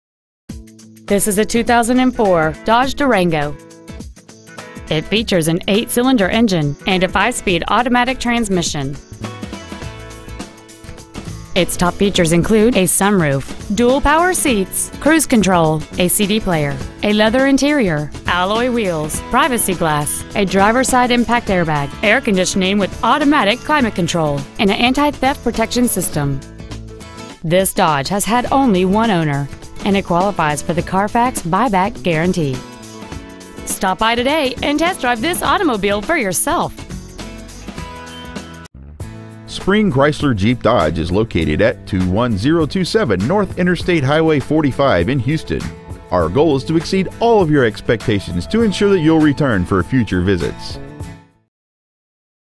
2004 Dodge Durango Tomball TX
Year : 2004

 Make : Dodge

 Model : Durango

 Engine : 8 Cyl.

 Trans . : Automatic

 Exterior : Bright White

 Miles : 126,431

 Interior : Medium Slate Gray

 Stock : C594704A

 21027 N. IH 45

 Pre-Owned 2004 Dodge Durango Houston TX